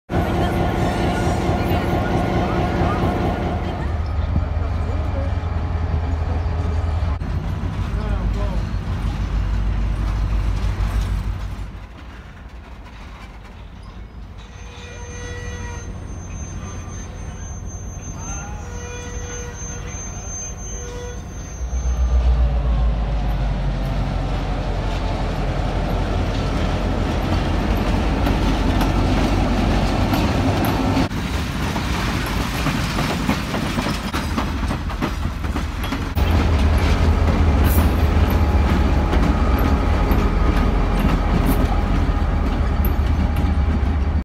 Polar Express Train Ride in Bryson City North Carolina
Take an extraordinary train ride to the North Pole, and embark on a journey that shows you that the wonder of life never fades for those who believe. The Polar Express Train Ride departs daily from Bryson City with Great Smoky Mountains Railroad through December 31. Tickets and information